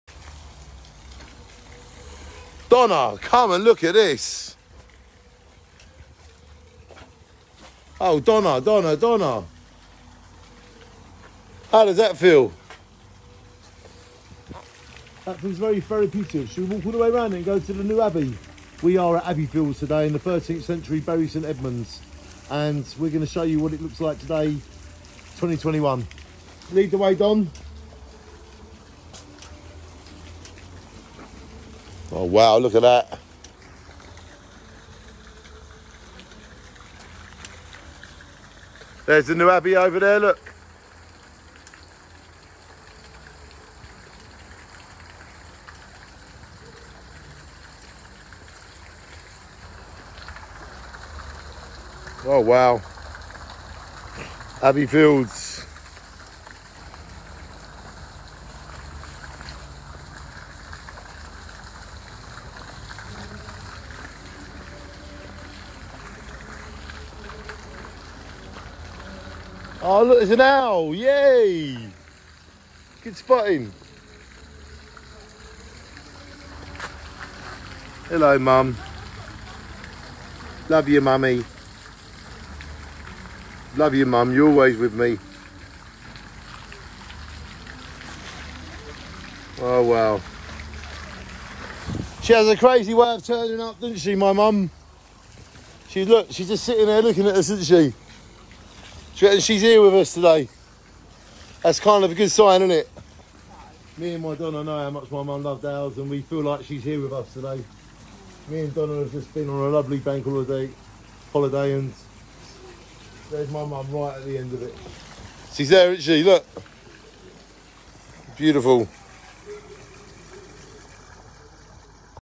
13th century Abbey Fields Bury Saint Edmunds bank holiday weekend 2021 I’m right at the end of this
13th century Abbey Fields Bury Saint Edmunds bank holiday weekend 2021 I’m right at the end of this film we see you now which means my mum is with us￼